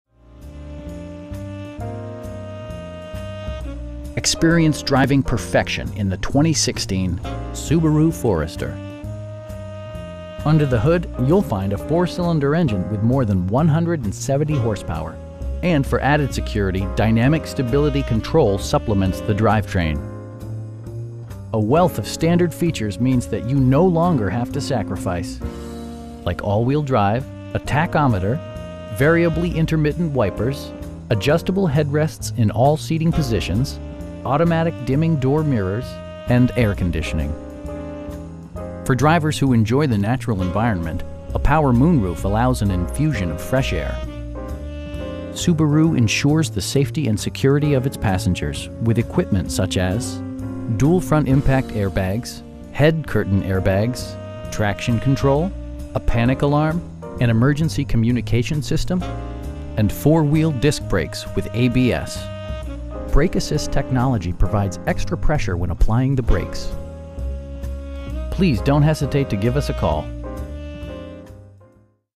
2016 Subaru Forester 2.5i Premium in Salem, OR 97301
Honda of Salem
1101 Van Ness Ave NE

Experience driving perfection in the 2016 Subaru Forester. Under the hood you'll find a 4 cylinder engine with more than 170 horsepower, and for added security, dynamic Stability Control supplements the drivetrain. A wealth of standard features means that you no longer have to sacrifice. Like all wheel drive, a tachometer, variably intermittent wipers, adjustable headrests in all seating positions, automatic dimming door mirrors, tilt steering wheel, power door mirrors, and air conditioning. For drivers who enjoy the natural environment, a power moon roof allows an infusion of fresh air. Subaru ensures the safety and security of its passengers with equipment such as: dual front impact airbags, head curtain airbags, traction control, a panic alarm, an emergency communication system, and 4 wheel disc brakes with ABS. Brake assist technology provides extra pressure when applying the brakes. You will have a pleasant shopping experience that is fun, informative, and never high pressured. Please don't hesitate to give us a call.